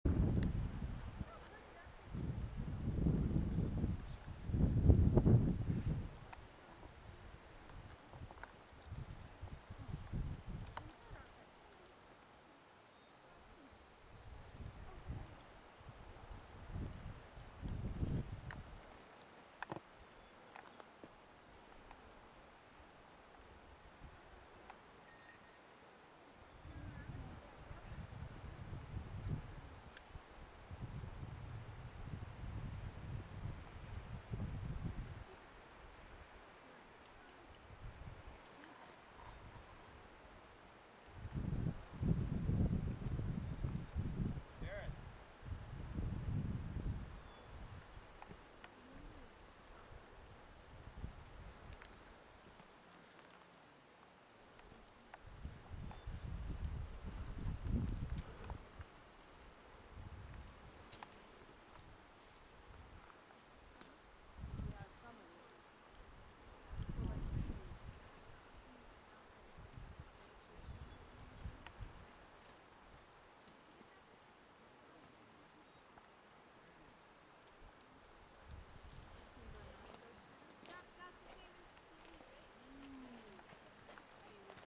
Burstall Pass
The view from the top of Burstall Pass in  Peter Lougheed Provincial Park